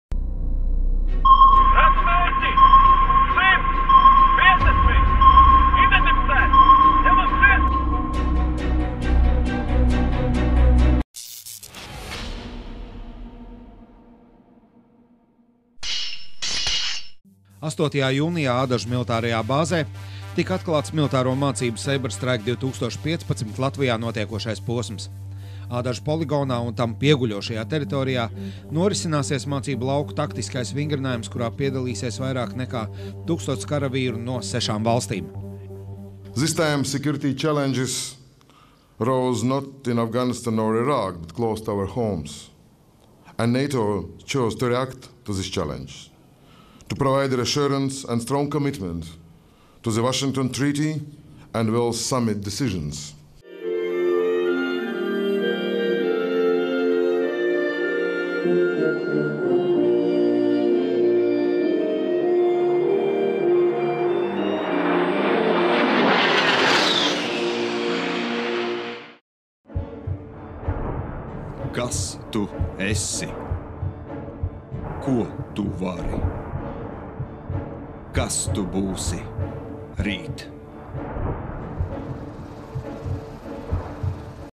Ādažos atklātas mācības "Saber Strike" 2015
Pirmdien, 8.jūnijā, plkst.10  Ādažu bāzē norisinājās starptautisko mācību „Saber Strike 2015” svinīgā atklāšanas ceremonija, kurā piedalījās Nacionālo bruņoto spēku komandieris, ģenerālleitnants Raimonds Graube.

Atklāšanas pasākumā Baltijas bataljonu pārstāvēja tajā iesaistītie Latvijas, Lietuvas un Igaunijas karavīri. Britu vadīto Ziemeļvalstu bataljonu pārstāvēja Somijas, Norvēģijas, Amerikas Savienoto valstu (ASV) un Lielbritānijas vienības. Parādē piedalījās arī NBS Štāba bataljona karavīri un Zemessardzes 27.kājnieku bataljona zemessargi, kuri mācībās iejutīsies nosacītā pretinieka lomā.  No militārās tehnikas bija apskatāmas divas Somijas bruņumašīnas ar mīnmetējiem. Pāri atklāšanas ceremoniju pārlidoja arī divas ASV Gaisa spēku lidmašīnas A-10C, kas sākot no 15.-19.jūnijam veiks zemos lidojumus Latvijas gaisa telpā.

Ādažu poligonā un tam pieguļošajā teritorijā 8.jūnijā sākas lauka taktiskais vingrinājums, kurā piedalīsies vairāk nekā 1000 karavīru. Vingrinājuma laikā notiks arī Baltijas bataljona nacionālā līmeņa sertificēšana dalībai NATO Reaģēšanas spēkos.

No 8. līdz 19.jūnijam Baltijas valstīs un Polijā norisinās militārās mācības „Saber Strike”, kurās piedalās vairāk nekā 6000 karavīru no Kanādas, Dānijas, Igaunijas, Somijas, Vācijas, Latvijas, Lietuvas, Norvēģijas, Polijas, Portugāles, Slovēnijas, Lielbritānijas un ASV.

 „Saber Strike” ir ikgadējas ASV vadītas apvienotās sauszemes un gaisa spēku mācības, kas tiek organizētas kopš 2010.gada. Tās ir iekļautas arī NATO vadīto mācību sērijā „Allied Shield”.

On June 8 in Adazi Training Area, Latvia the opening ceremony for the international exercise "Saber Strike" took place. Chief of Defence of Latvia Lt.Gen.Raimonds Graube delivered the opening speech.

Soldiers from Estonia, Finland, the Great Britain, Latvia, Lithuania, Norway and the US participated in the opening ceremony. The fly-over above the parade ground was performed by A-10.